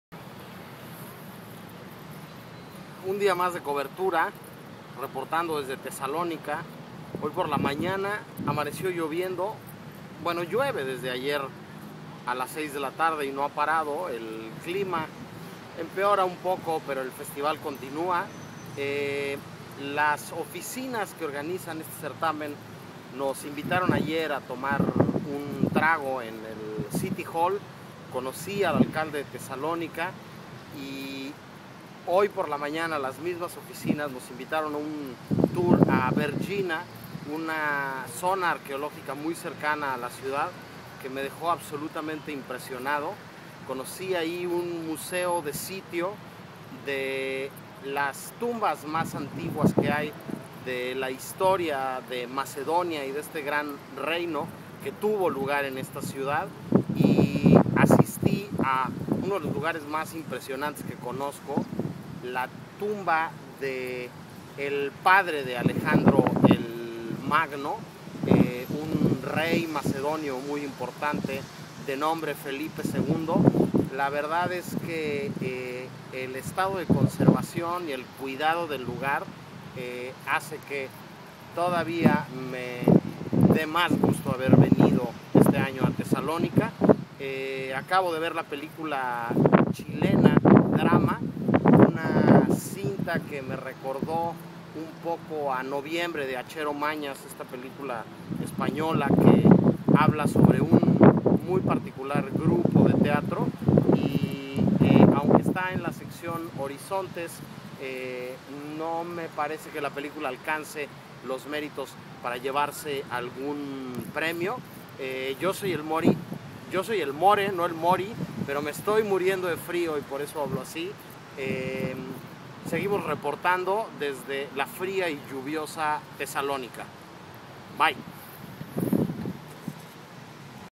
Toma 10:Llega la lluvia
Regresando de visitar tumbas